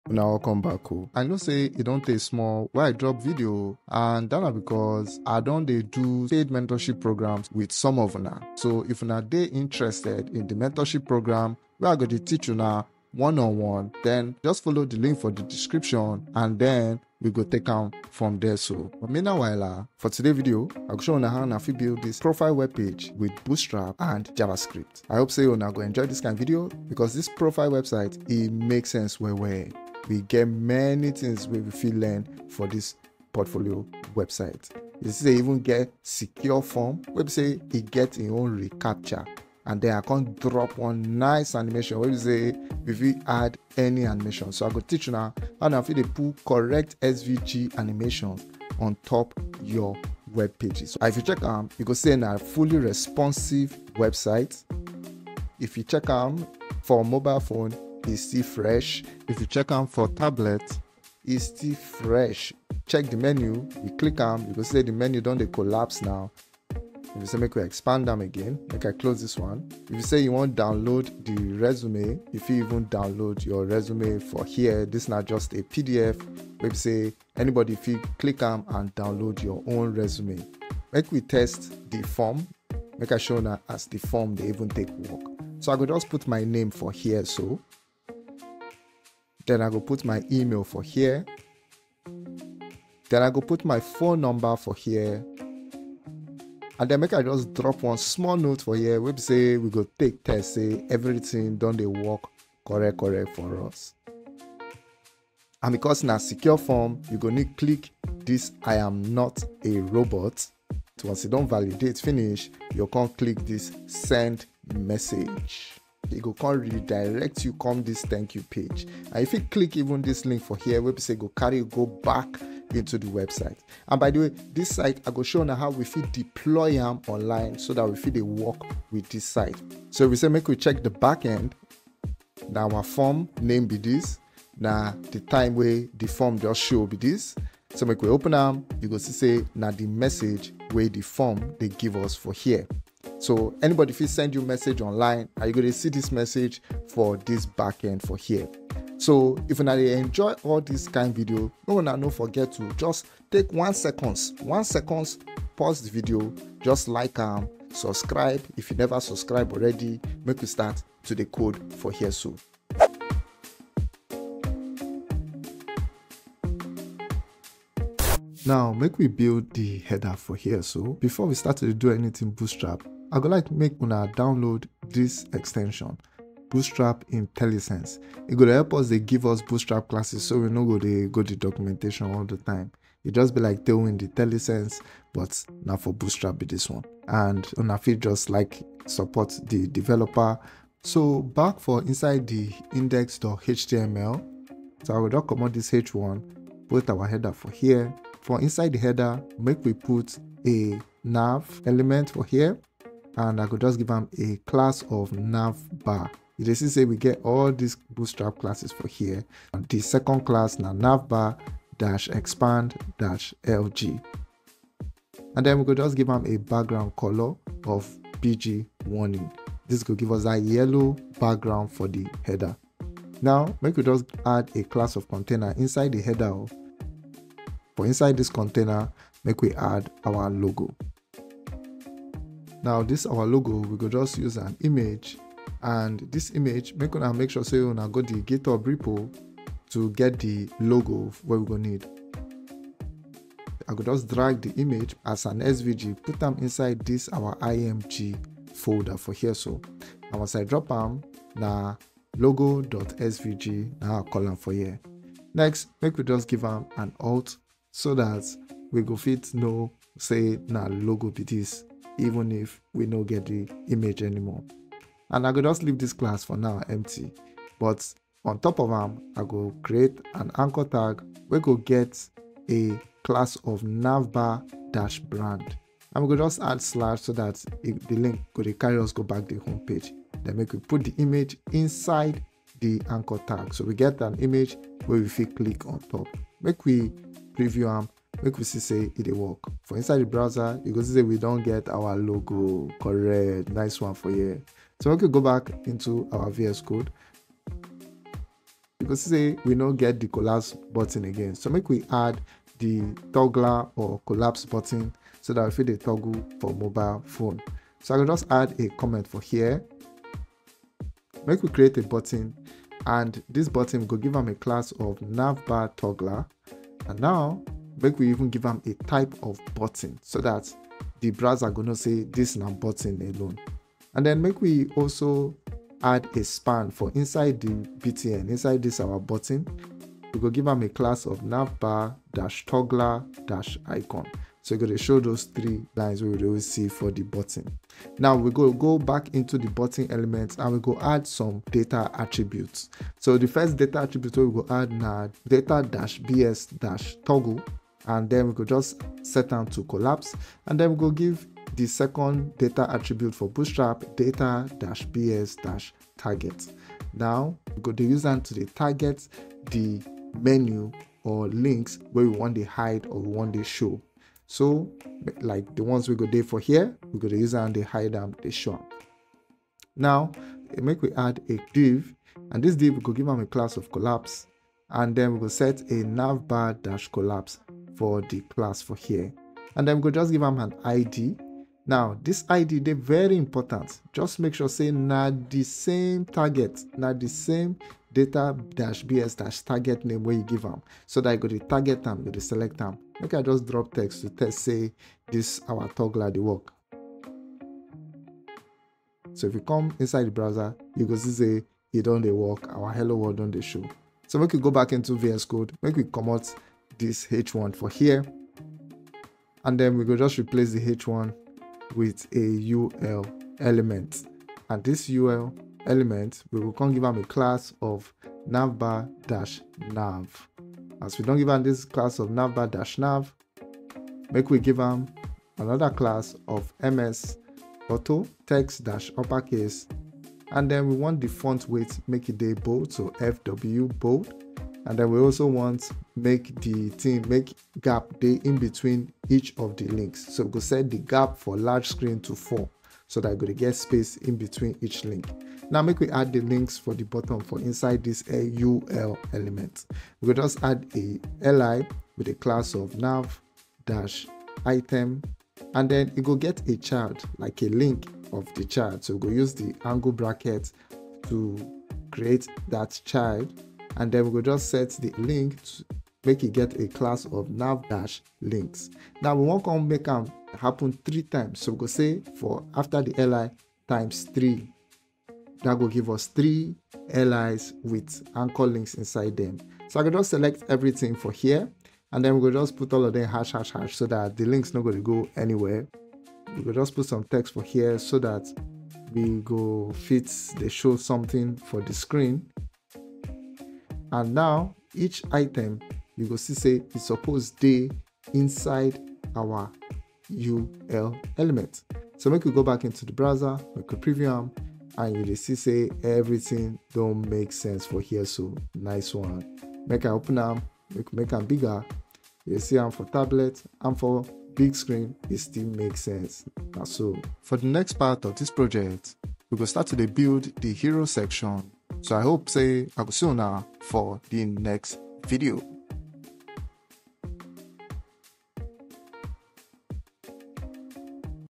Build a Responsive Profile Website with Bootstrap 5 & JavaScript Part 2 - Setup [Pidgin English]
For this video we go build and deploy one portfolio project with di latest Bootstrap 5. Dis project go show you how to add Lottie Animation, Javascript Dom Manipulation, Netlify. You go also learn how to add downloadable files and collect emails from your form inputs. And plenti plenti more dey inside.

Links used in this video :
Happy Learning!